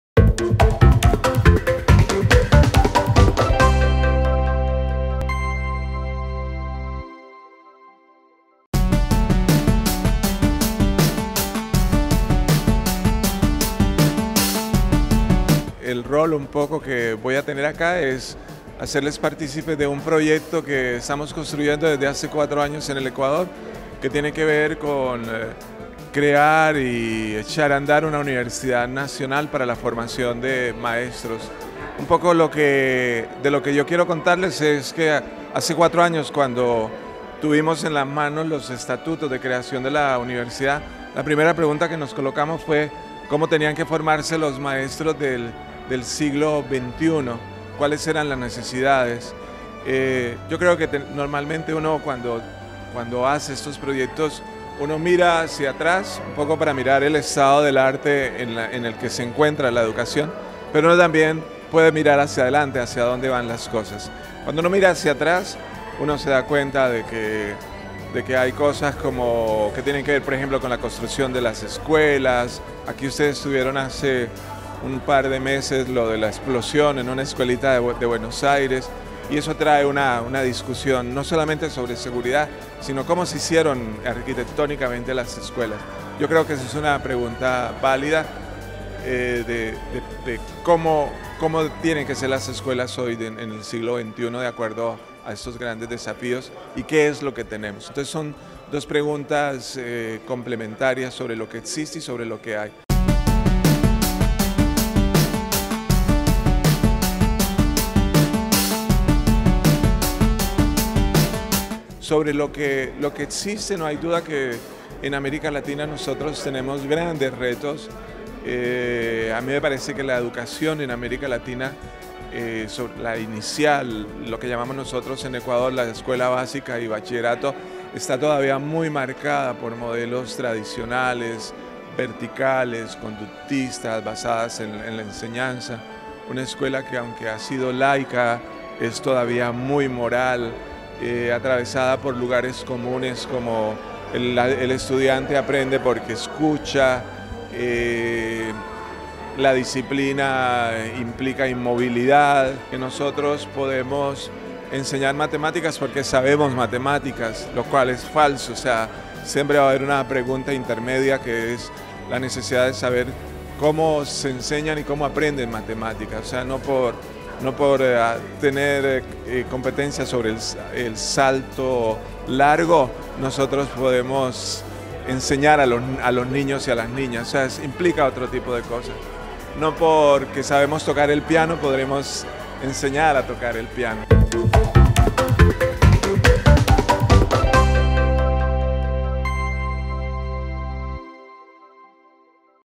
La UNLAR promueve 1ras Jornadas Nacionales de Educación- Freddy Álvarez
Debaten sobre innovación educativa, en el marco de las 1° Jornadas Nacionales de Educación que fueron organizadas por la UNLaR,  el Ministerio de Educación  y la UNIPE. Te mostramos las reflexiones de lxs principales referentes que participaron.